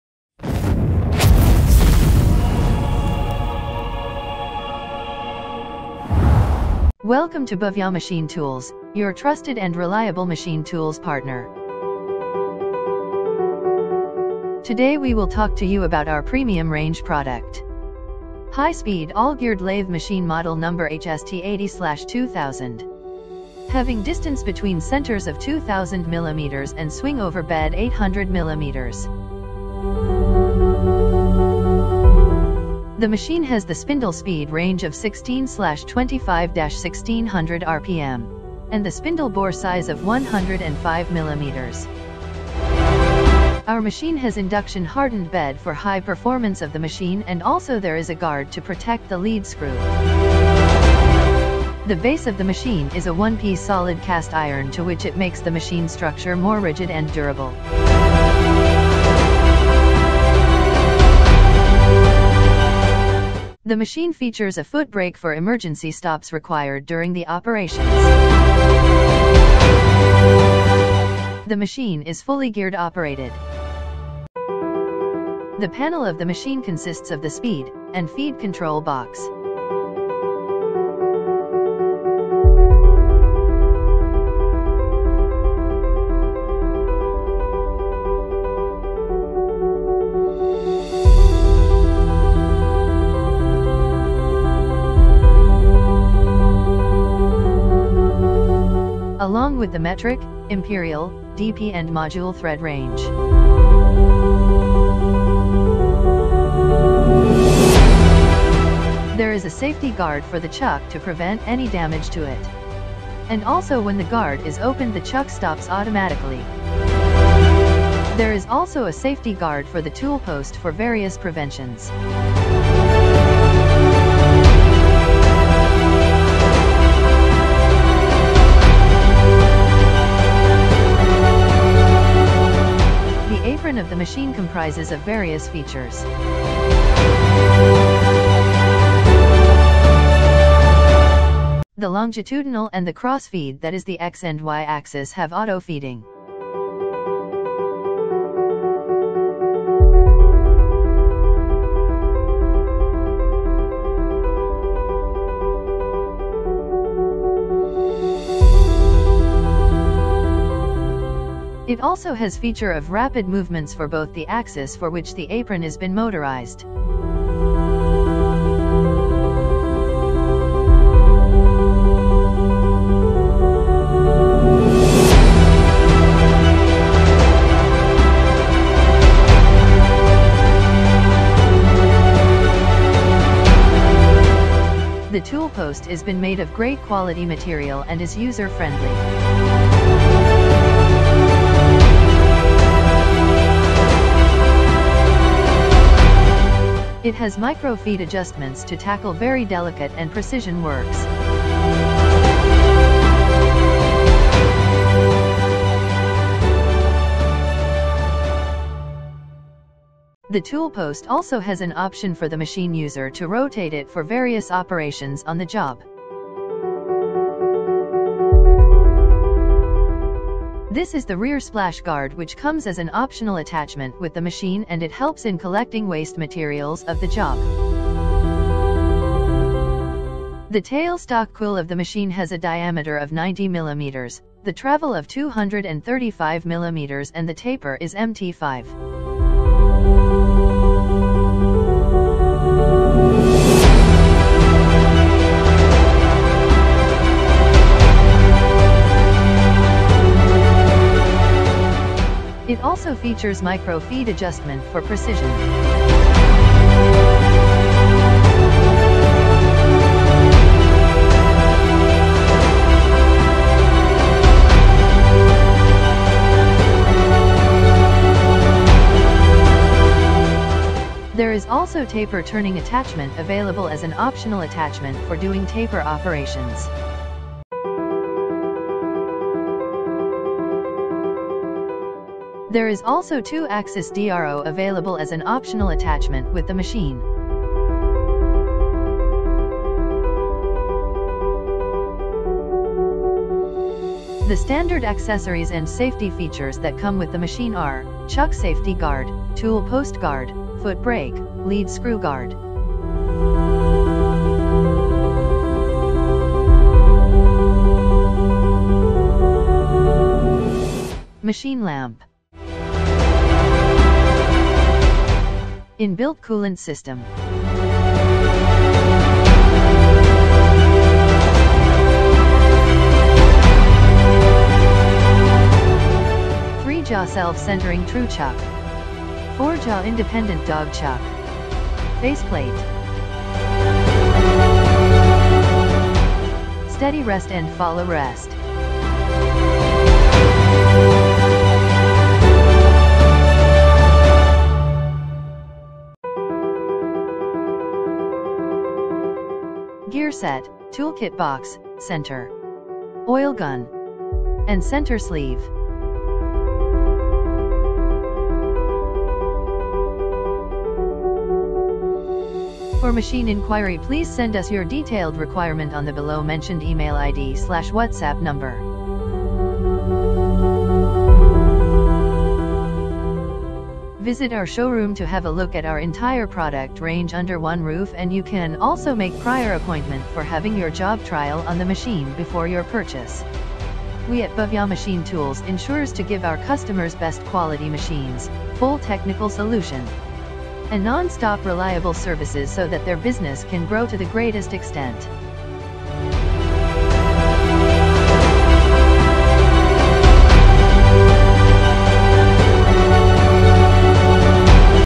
High Speed All Geared Lathe Machine (Model – HST80/2000) – Bhavya Machine Tools
The High Speed All Geared Lathe Machine (Model – HST80/2000) has distance between centres 2000mm & swing over bed of 800mm. The All Geared Lathe Machine has a spindle speed range 16/25-1600RPM & Spindle Bore size 105mm. This Lathe Machine is a one-piece solid cast iron machine base that makes the machine structure more rigid and durable. This HST80/2000 has an induction hardened bed for high performance and also has a lead screw guard for the protection. The high-speed lathe machine features a foot brake for emergency stops requirement for operations. The All Geared Lathe HST80/2000 machine is fully geared operated. The panel of the lathe machine consists of speed and feed control box. Along with the metric, imperial, DP & module thread range is also present. There is a safety guard for the chuck to repair any damaged tool and also when the guard is open the chuck stops automatically. There is also a safety guard for the tool post for various preventions.  The apron of HT66 machine comprises of various features. The longitudinal and cross feeds i.e. X & Y axis have auto feeding facilities. It also has the rapid movement feature for the apron that is been motorized.

The tool post is made of great quality material and is user friendly. It is due to this reason the HST 80/2000 machines give precision in heavy-duty. It has micro feed adjustments to tackle very precision and delicate works. The tool post also has the option for the user to rotate for various operations on the job. There is a rear splash guard that comes as an optional attachment with the machine and helps in collecting waste materials in the job.

The tail stock quill has a diameter of 90mm with a travel of 235mm and the taper is MT-5. It also has a micro feed feature for feed adjustments for high precision and can move left and right for 10mm distance. Taper turning attachment is also available as an optional accessory for doing taper operations. There is also 2 Axis DRO available as an optional accessory. The standard accessories and features that come with the lathe machinery are are chuck safety guard, lead screw guard, foot brake, machine lamp, inbuilt cooling system, toolkit box and many more needed requirements.

To know more about High Speed All Geared Lathe Machine,